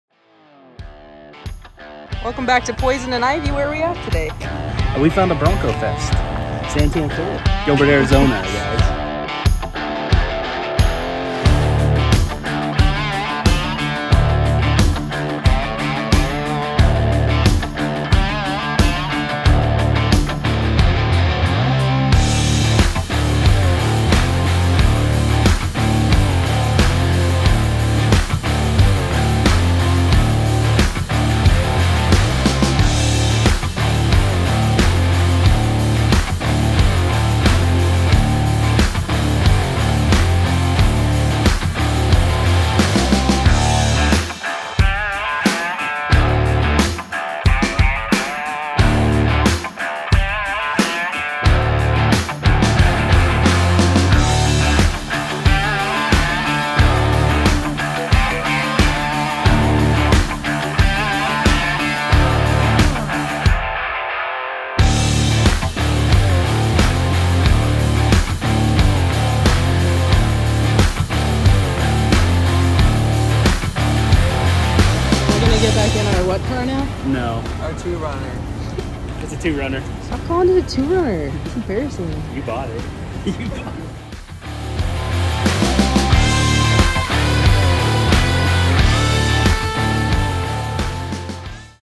Bronco Wishes and Coffee Dreams... San Tan Ford, Gilbert, Arizona
First Meet of Broncos and Coffee at San Tan Ford - Gilbert, Arizona! Thanks to Arizona Broncos Facebook Group for setting it up - super cool to see all of the Broncos rally... enjoy the video!